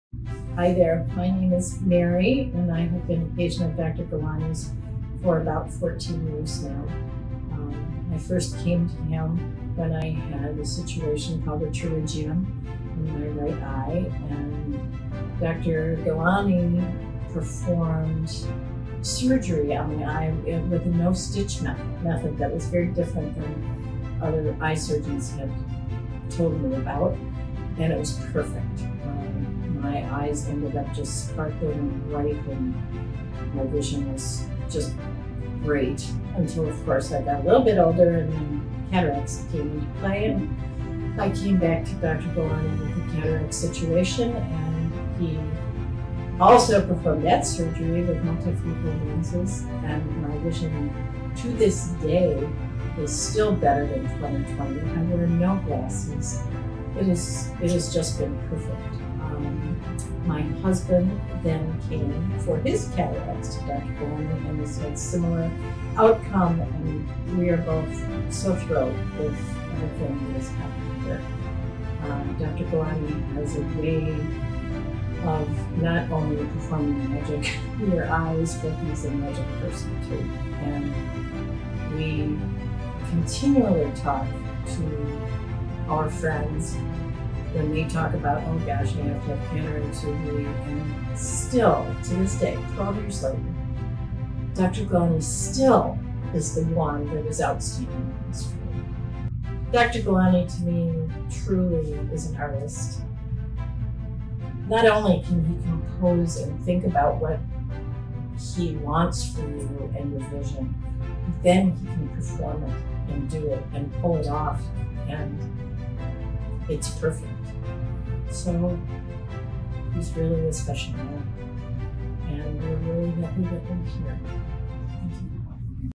ARTIST for your EYES! Combined LenzOplastique+ LaZrplastique + Amniotic Surgery to 20/20
Mary was referred to Dr.Gulani 15 years ago for Advanced Pterygium that he artistically removed with his No Stitch Technique and then when she developed age-related Cataracts in later years, he performed Multifocal Cataract surgery (LenzOplastique)  with LaZrPlastique and brought her to 20/20 vision at distance and near without glasses.
In this video, 15 years later, she is describing her and her husband's experience at GulaniVision